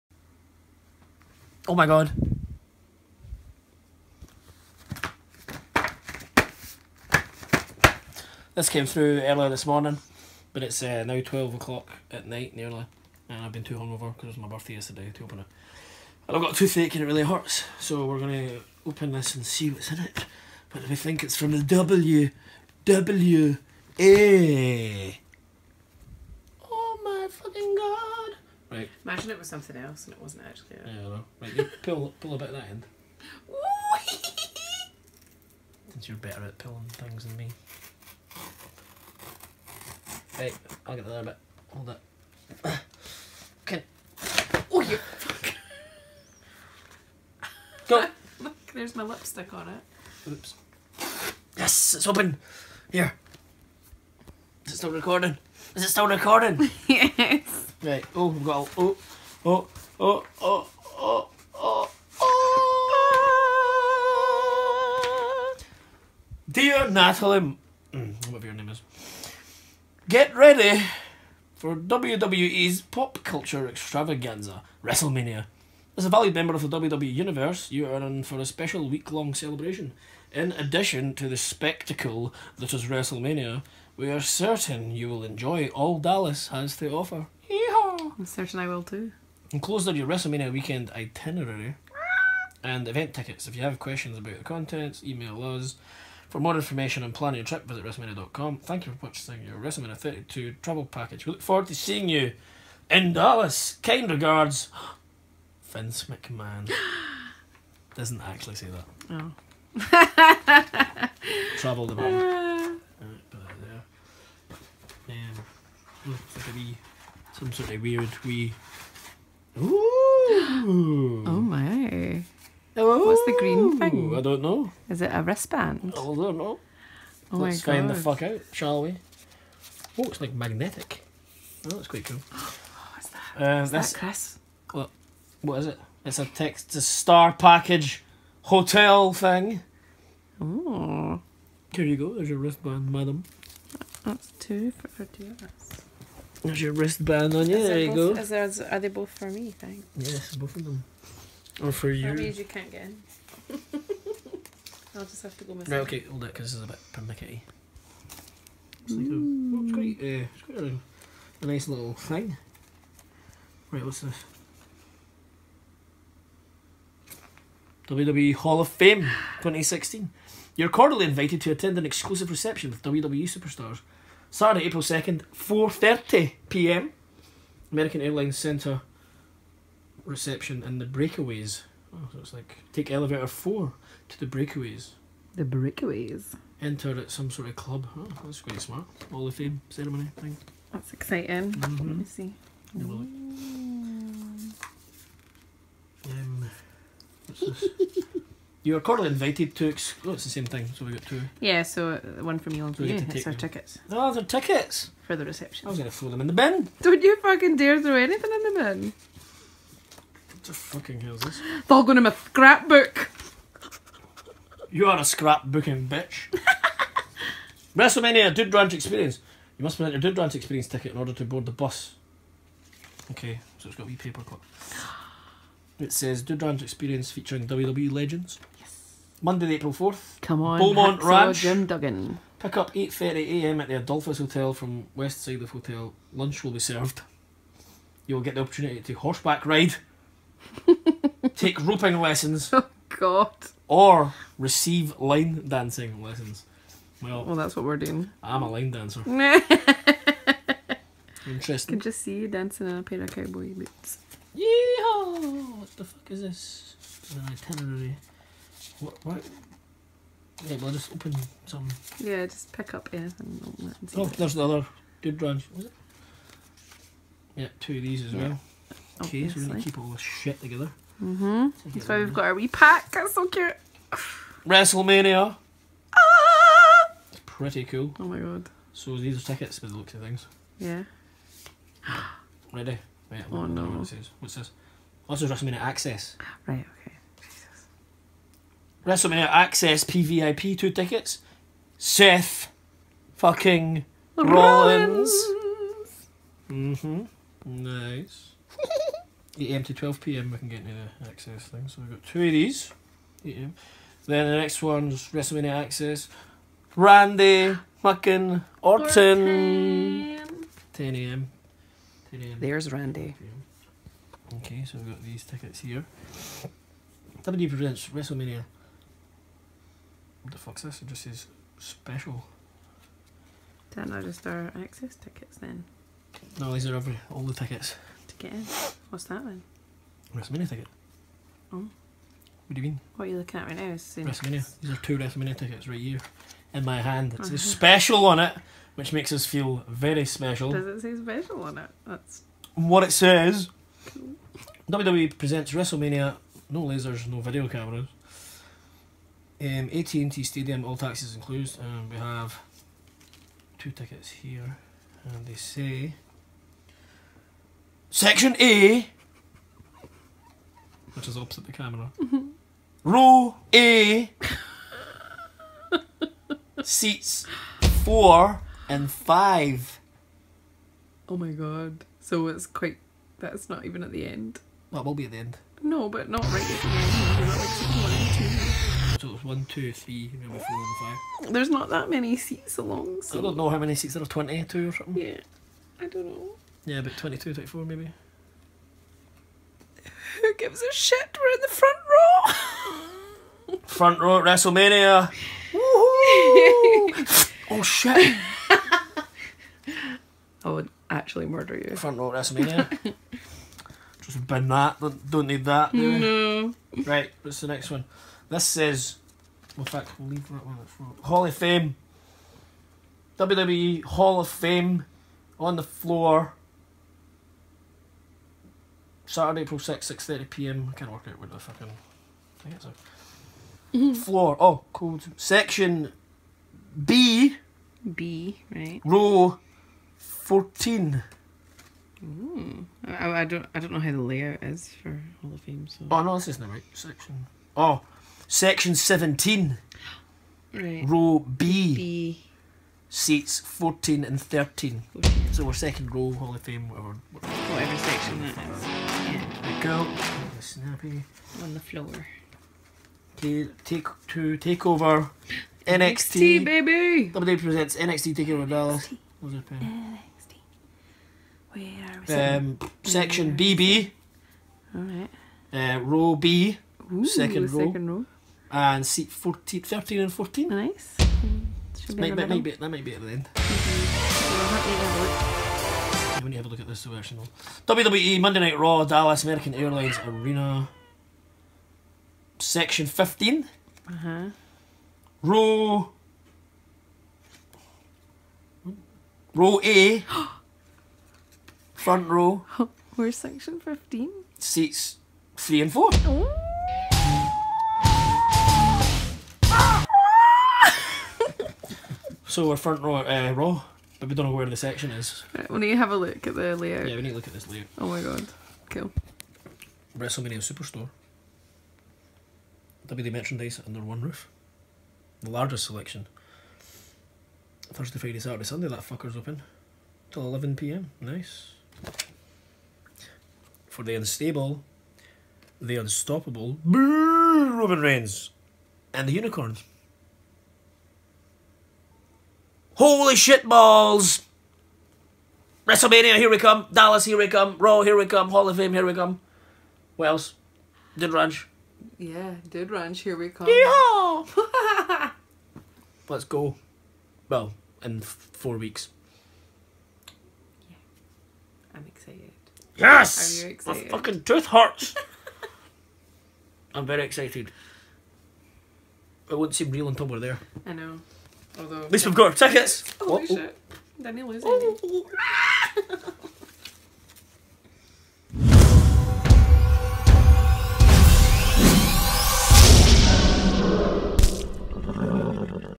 UNBOXING MANIA 32!!!
The lady and I received our Wrestlemania 32 tickets in the first week of March 2016.

We thought we would "unbox" that motherfucker.